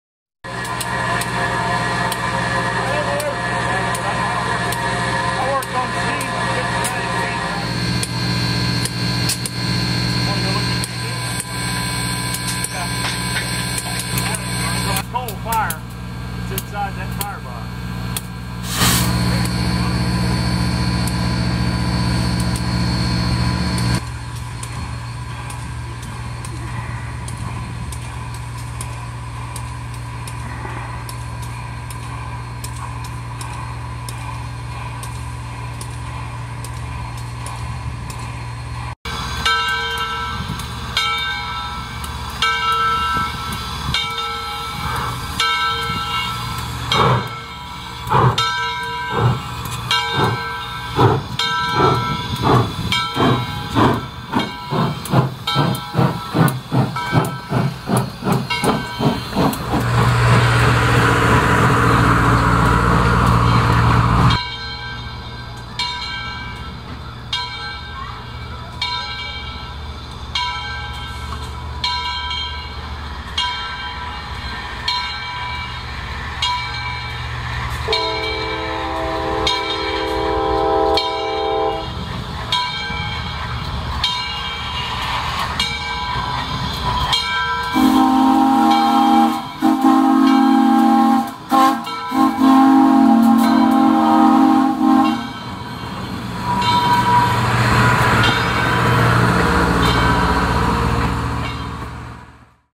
Southern 630 in Chattanooga
Southern 630 steam locomotive and scenes at Tennessee Valley Railroad, Chattanooga TN